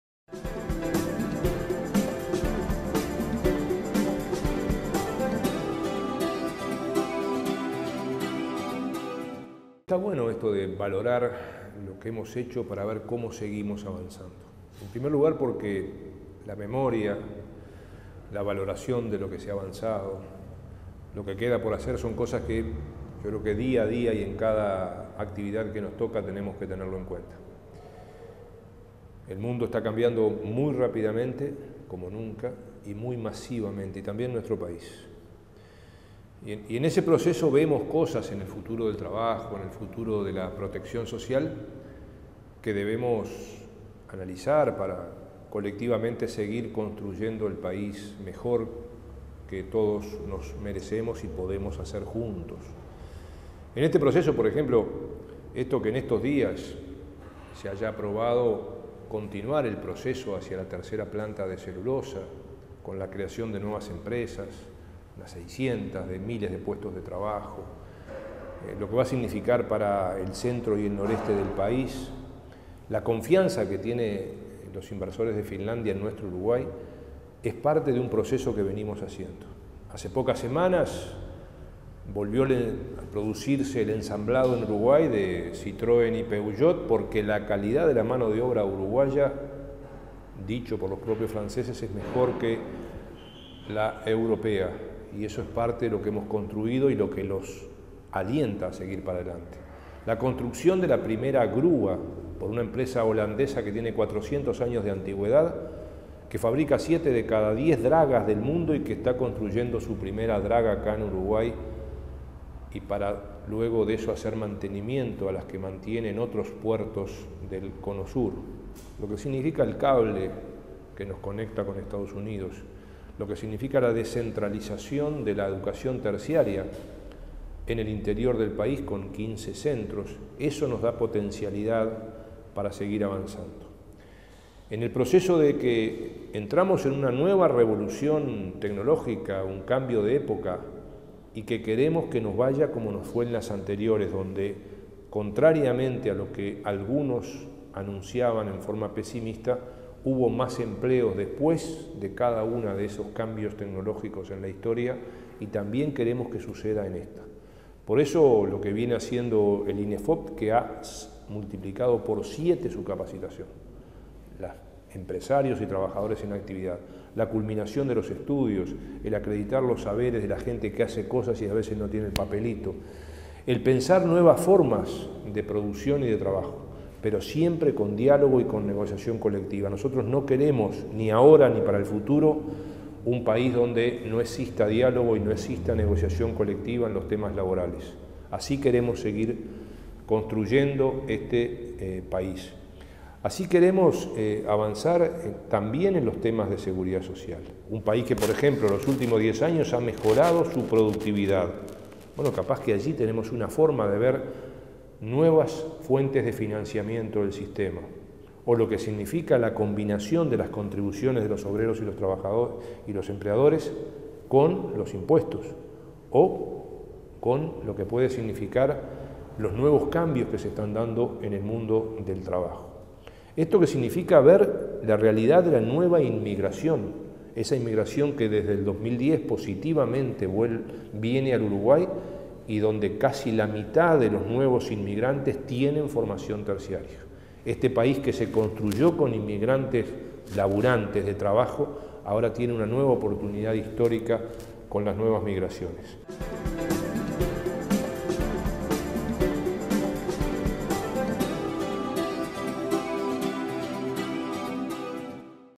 Transformaciones, Ministro de Trabajo y Seguridad Social, Ernesto Murro
Ministras y ministros nos dan su perspectiva sobre Transformaciones en nuestro país.
Hoy, el ministro de Trabajo y Seguridad Social, Ernesto Murro, se expresa sobre las nuevas oportunidades históricas que el Uruguay está viviendo a partir de distintas inversiones del extranjero y del arribo de inmigrantes, que demuestran la confianza en el país.